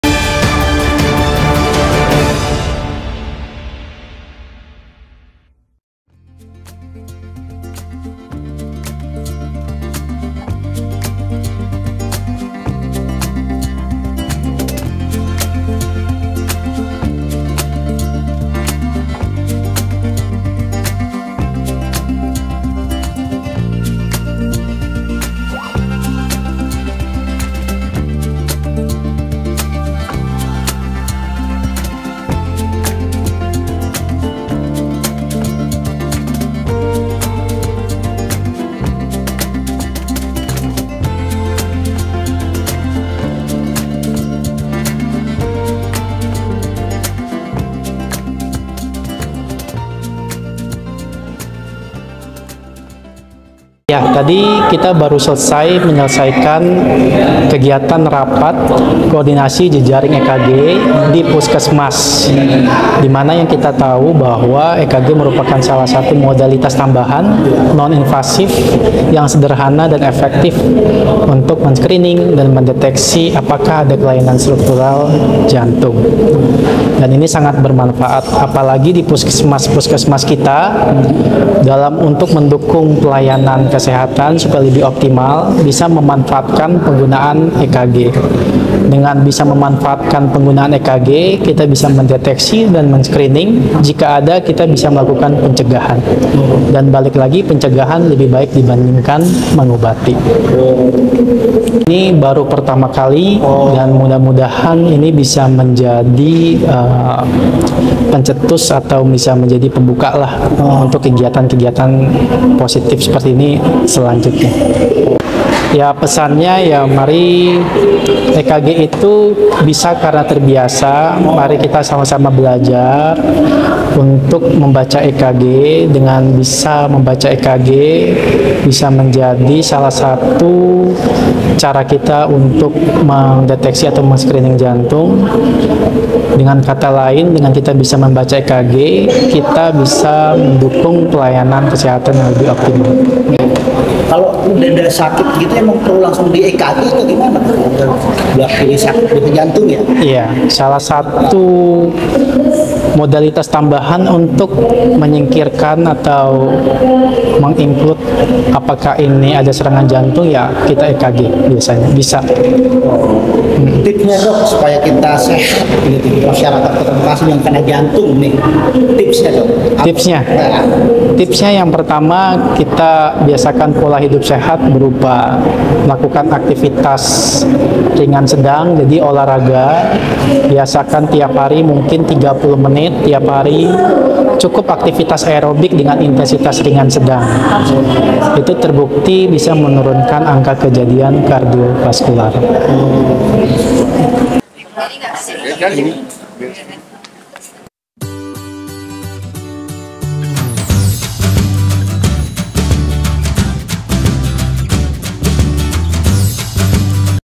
Dokter Spesialis Jantung & Pembuluh Darah RSUD Jatisampurna Memberikan Edukasi di Dinkes Kota Bekasi
BEKASIPEDIA TV - Dinas Kesehata Kota Bekasi terus melakukan inovatif dan peningkatan kualitas pelayanan kesehatan untuk masyarakat Kota Bekasi.

Seperti diketahui Pemkot Bekasi melalui Dinas Kesehatan Kota Bekasi telah memfasilitasi 28 Puskesmas di Kota Bekasi dan 4 RSUD kelas D yang memiliki EKG.

Dengan adanya fasilitas tersebut, maka masyarakat Kota Bekasi yang memiliki gejala sakit jantung sudah bisa langsung terdeteksi sejak dini di Puskesmas.

Oleh karena itu, Dinas Kesehatan Kota Bekasi melaksanakan rapat kordinasi jejaring EKG untuk 28 Puskesmas dan 4 RSUD Tipe D agar makin lihai mengoperasikan dan membaca hasil EKG tersebut.

Kegiatan dilangsungkan pada Jumat (20/10/2023) di Lt 4 Kantor Dinas Kesehatan Kota Bekasi.

Sedangkan sebagai pembicara adalah dr. Farian Nicolas Sp.JP dari RSUD Jatisampurna.

Berikut liputannya bersama Bang Pede. Cekidot Serrrrr...